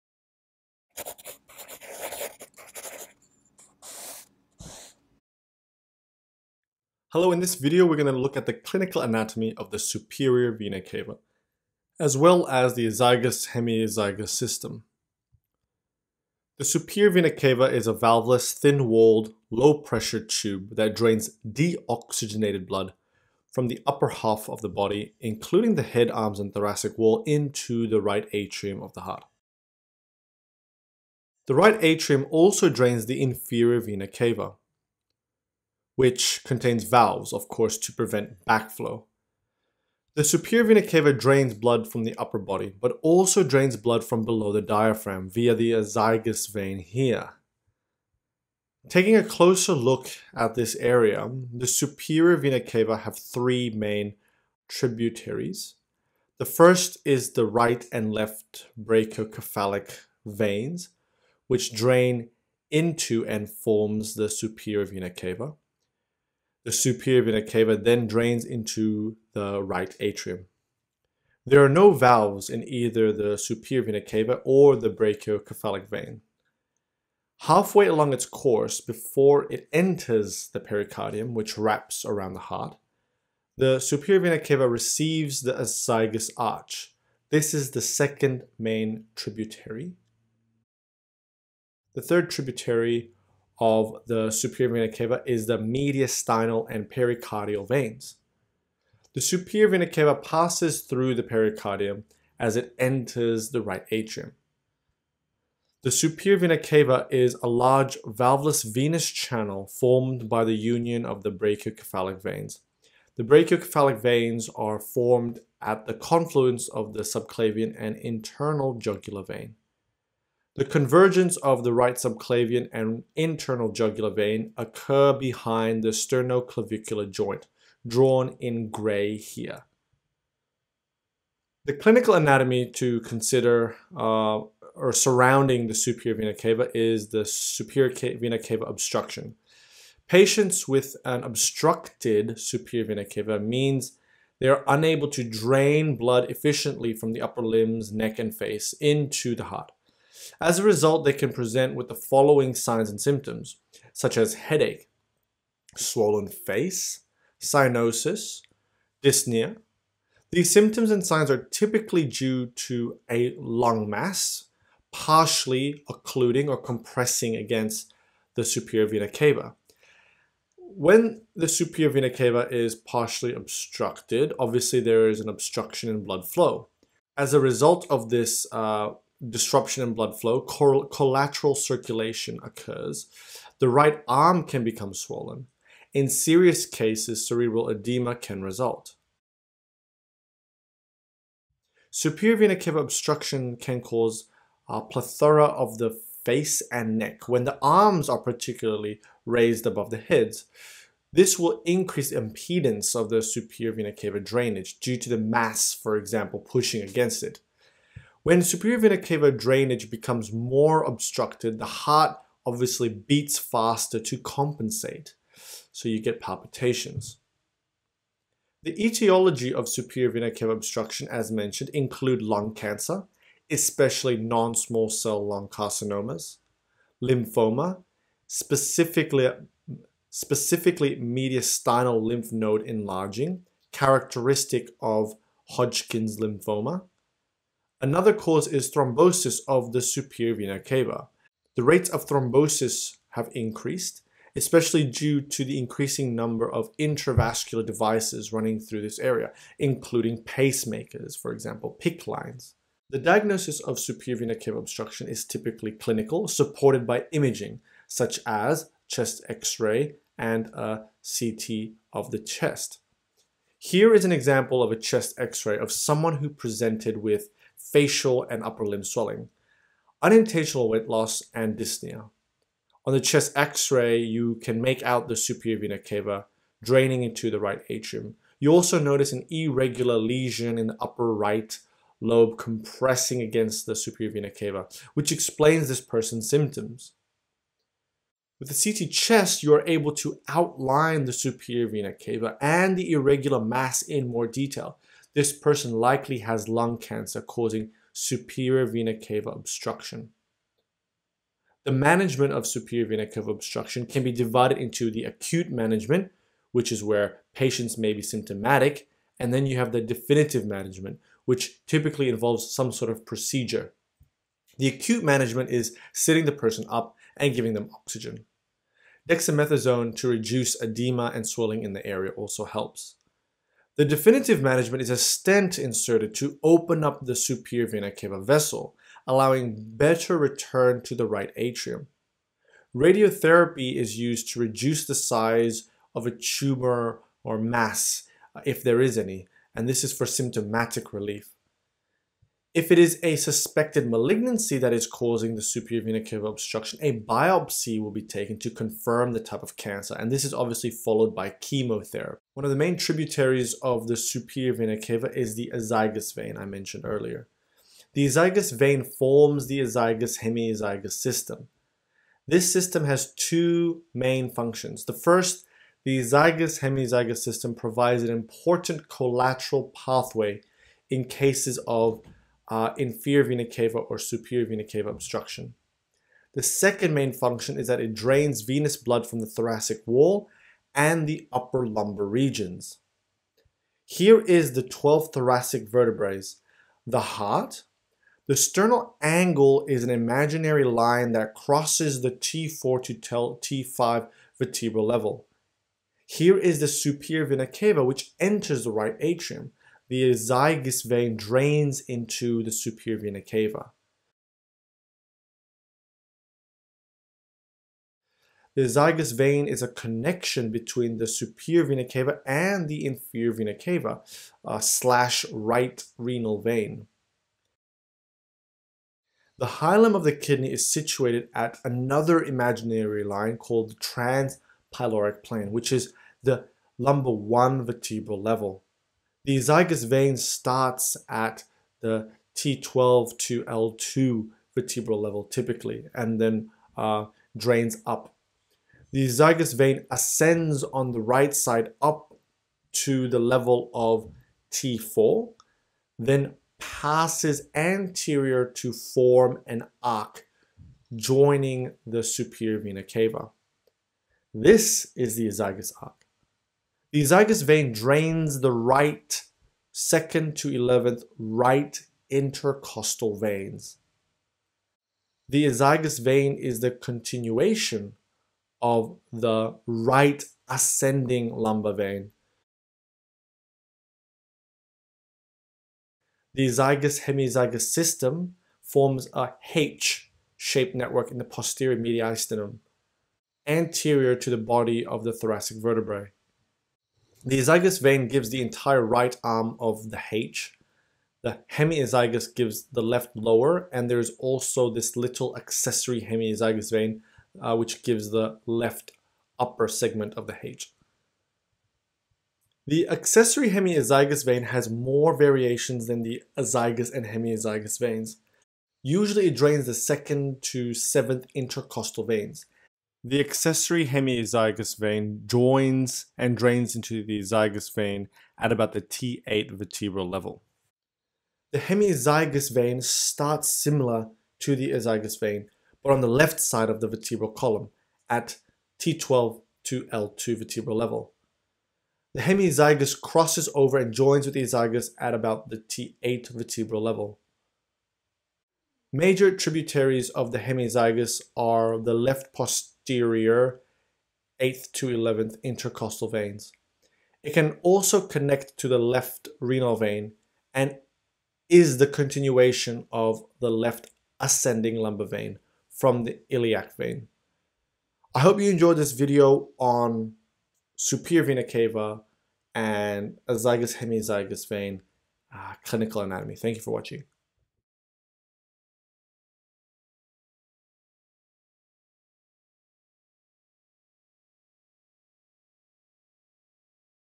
Superior vena cava and the azygos system clinical anatomy - SVC obstruction (oncology emergency)
Explore the clinical anatomy of the superior vena cava and azygos system, focusing on their roles in venous return to the heart. This video also explains the causes, signs, and collateral pathways involved in superior vena cava obstruction, a critical oncologic emergency.

🎥 DON'T JUST WATCH, LEARN ACTIVELY! TRY THE QUIZ! 🤓
👆Hit this quiz and show of your main character aura!